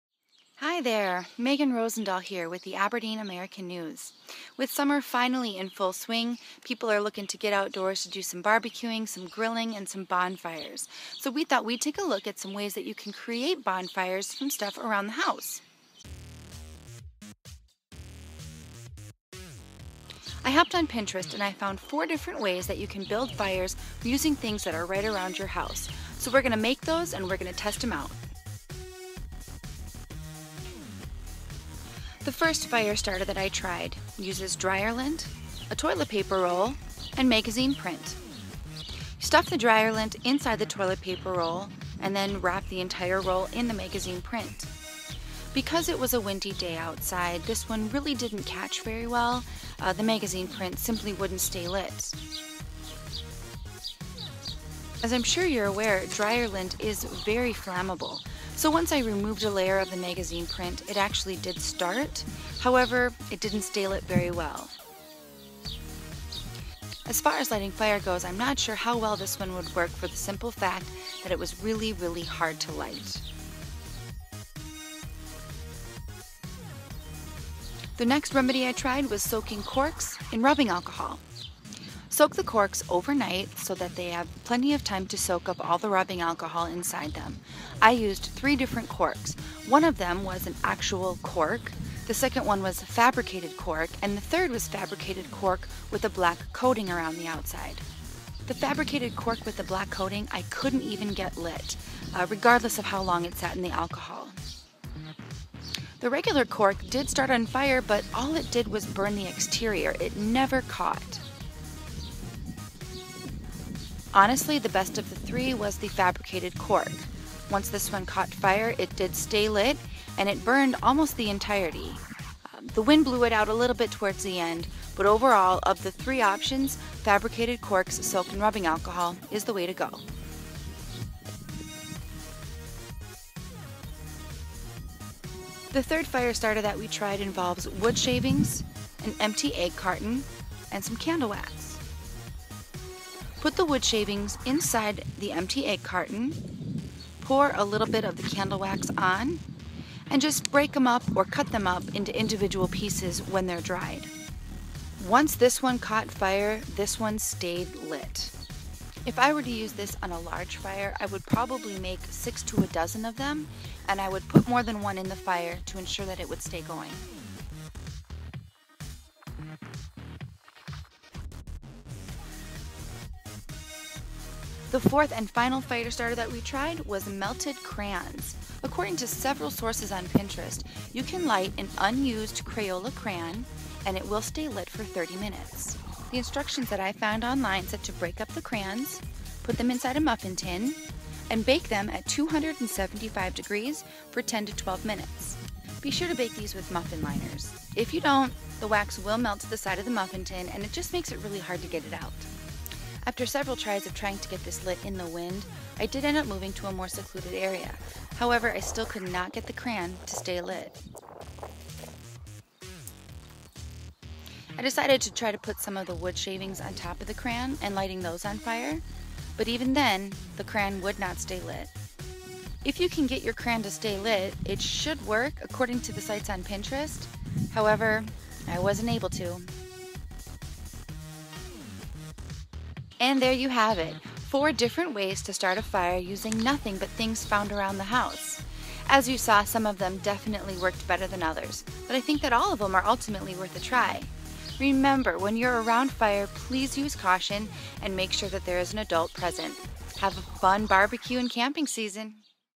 Testing out homemade bonfire starters
There are several ways to start bonfires using materials that you can find around your home. We found four different methods and we put them to the test. Check out the video to see which ones work and which ones failed.